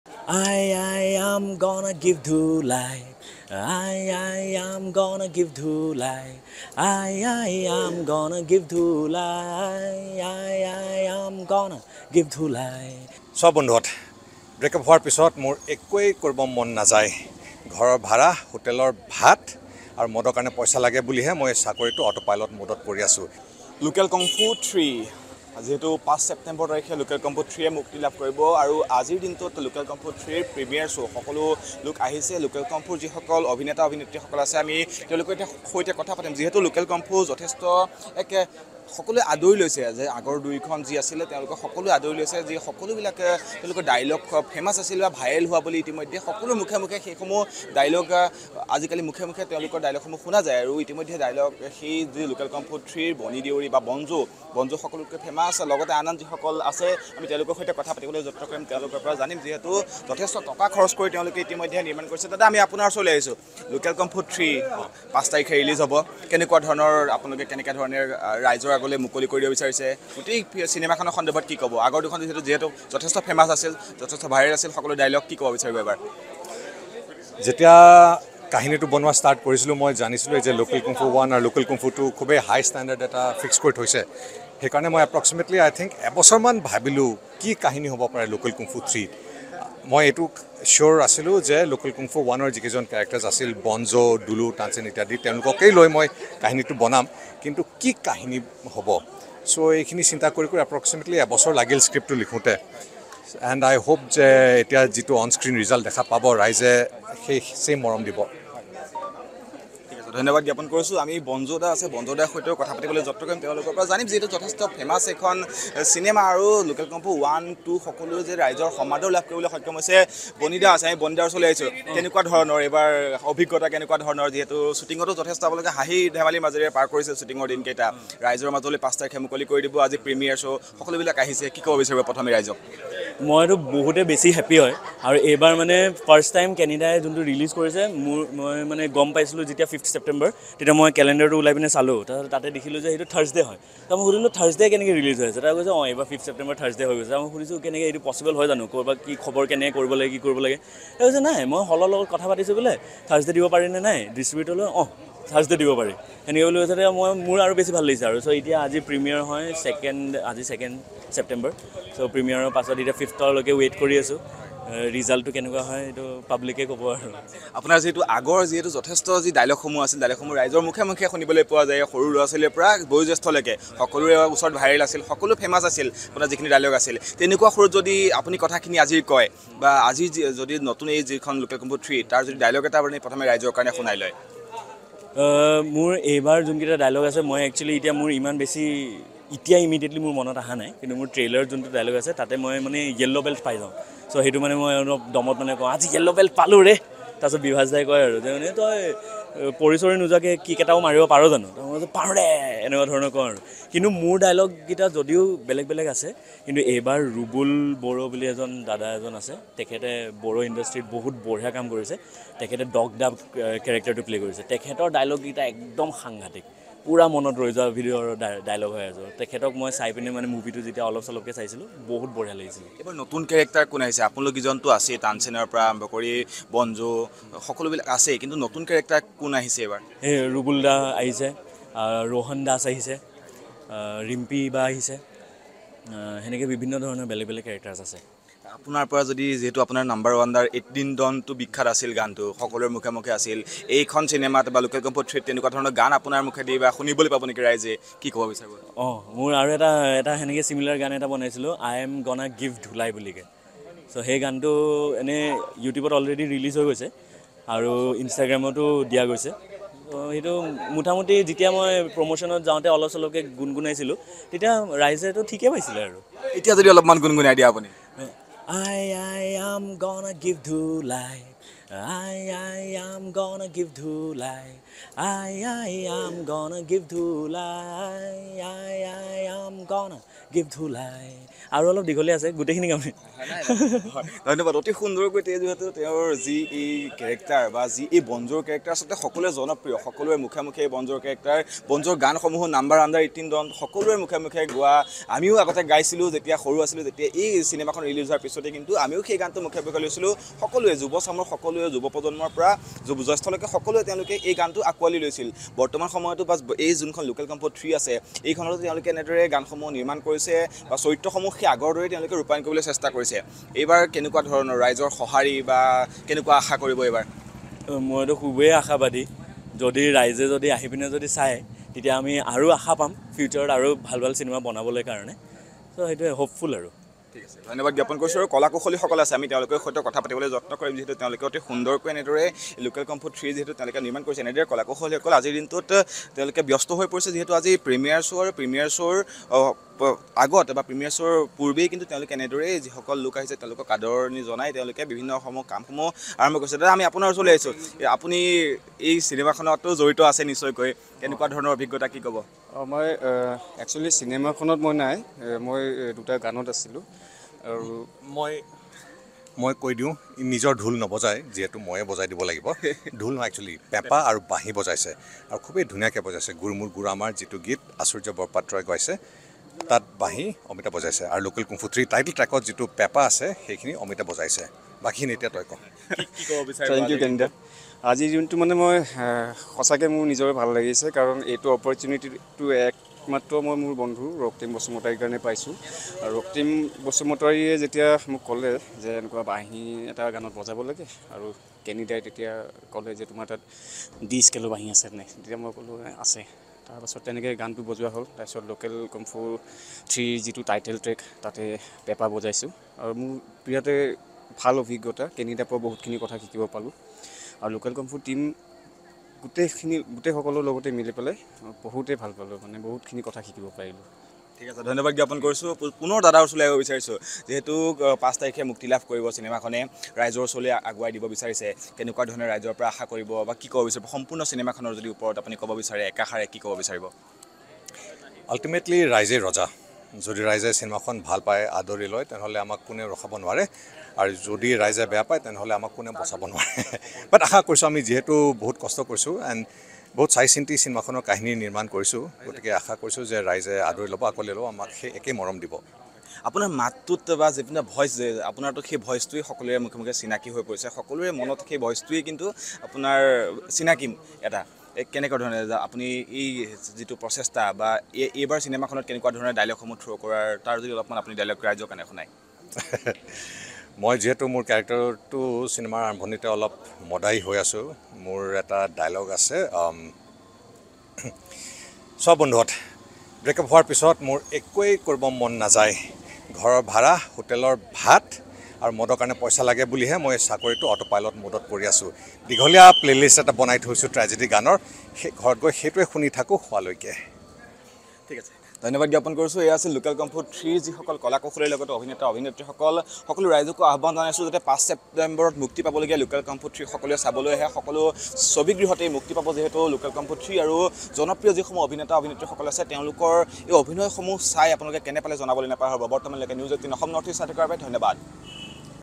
Local Kung Fu Part 3 Releasing on 5th September | ৫ছেপ্তেম্বৰত মুক্তি লাভ কৰিব লোকেল কুংফু। N18V
Local Kung Fu Part 3 Releasing on 5th September | ৫ছেপ্তেম্বৰত মুক্তি লাভ কৰিব লোকেল কুংফু। আজি মুকলি হ'ল প্ৰিমিয়াৰ শ্ব

লগতে চাওঁক দিনটোৰ বহু গুৰুত্বপূৰ্ণ খবৰ |